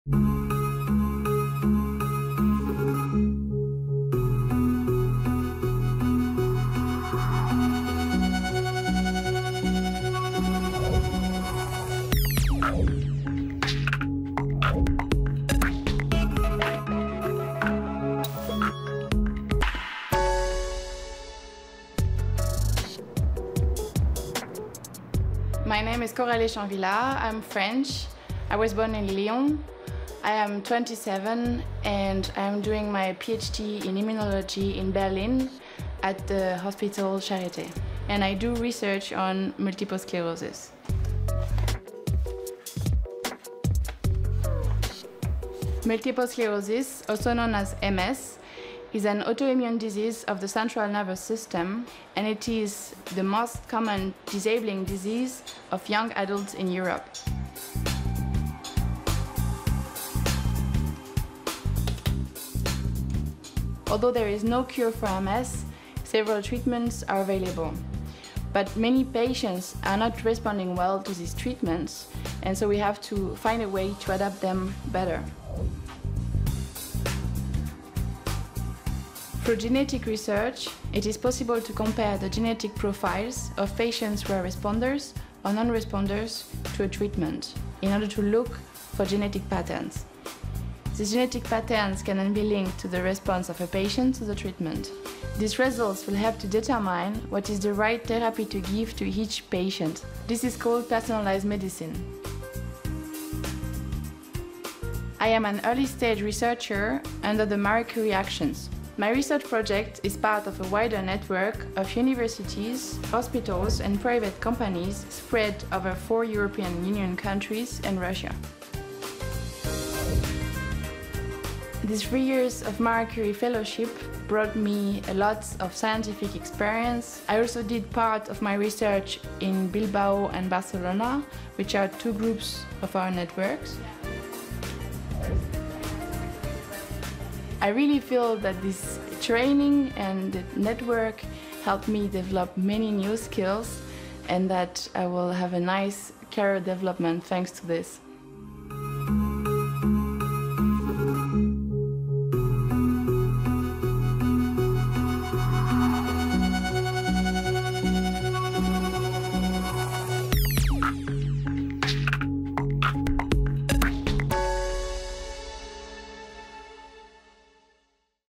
Gaining more scientific experience
Marie Curie Fellowships are European research grants available to researchers regardless of their nationality and field of research. In addition to generous fellowships, researchers have the possibility to gain experience abroad and in the private sector, and to complete their training with competences or disciplines useful for their careers. In this website you will learn more about those grants that promote excellence and innovation in Europe. Coralie Chanvillard is an early stage researcher undertaking a PhD in Berlin, under the Initial Training Network Grant of the Marie Curie programme. The Marie Curie grant gave her the opportunity to gain plenty of scientific experience and to do part of her research in other European Union Member States. She is researching her PhD in Immunology in Berlin at the Hospital Charité. Her research is on multiple sclerosis